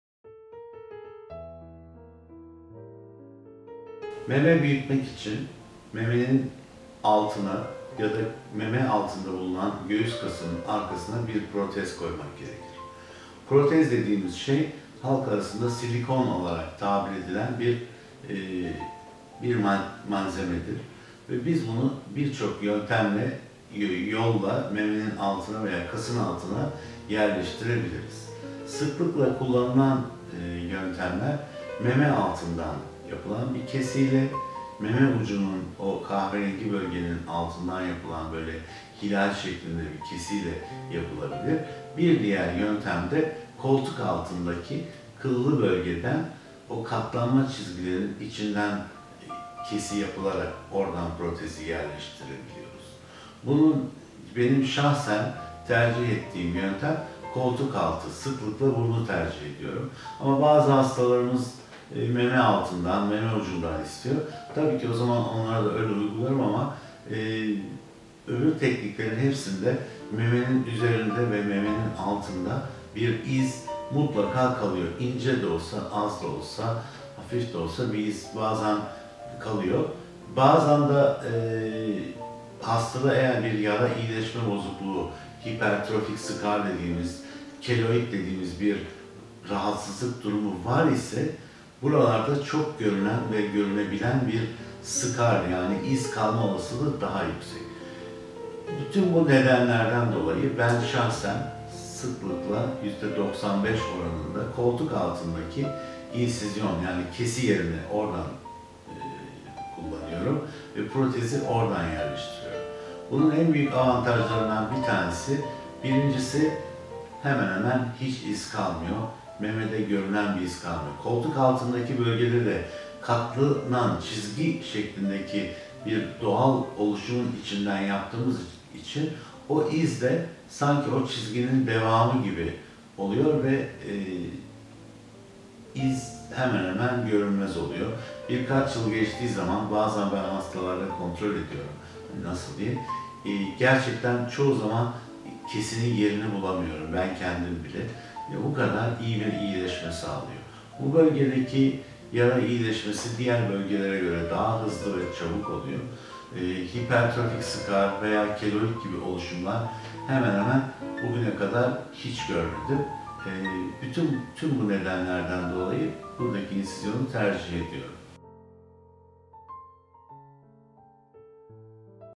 İzsiz göğüs büyütme ameliyatını nasıl yapıyorsunuz? Op. Dr. Nuri Battal
Estetik ve Plastik Cerrahi Uzmanı Op. Dr. Nuri Battal, izsiz göğüs estetiği ve izsiz göğüs büyütme ameliyatı hakkında deneyimlerini paylaşıyor.

Op. Dr. Nuri Battal, "İzsiz göğüs büyütme ameliyatını nasıl yapıyorsunuz?" sorusunu yanıtlıyor.